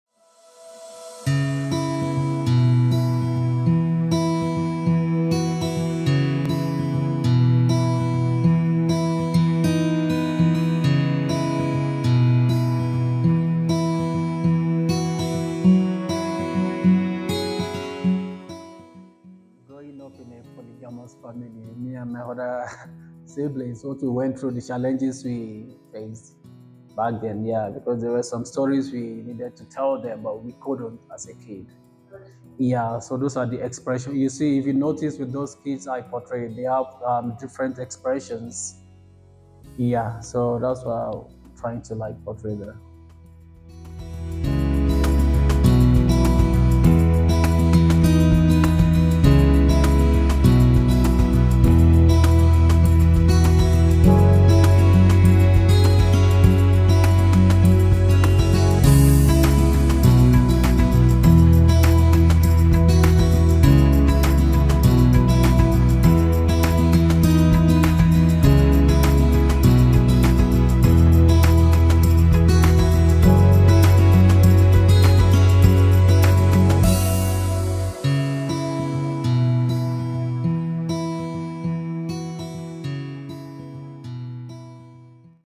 Sanusi Olatunji Spotlight
“Ideas were first explored, but in this moment dreams are being lived. Paint and paper safeguarding colours and African cultures on vivid collaged fabrics. Only time will tell the young about African queens and the long awaited victory at last.”

To our dedicated art collectors, note that these artworks by Sanusi Olatunji are the last works available in South Africa! So head to the Artyli Gallery in Nelson Mandela Square, Sandton, to add an exquisite work by Sanusi Olatunju to your art collection!

The ‘Roots to Wings’ exhibition concludes on the 25th of July, so stop by the Artyli Gallery to view this engaging exhibition currently showcasing artworks by esteemed contemporary African artists.